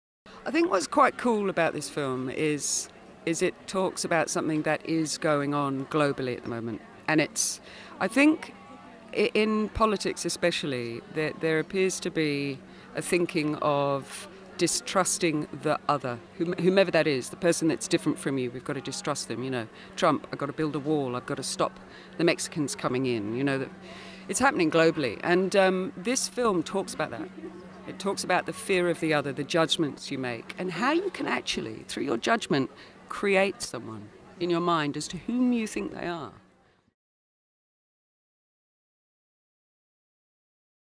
Rachael Blake at DIFF 2019 (snippet)
Australian actress Rachael Blake presented her latest feature "Slam" in Opening of the 10th Darwin International Film festival.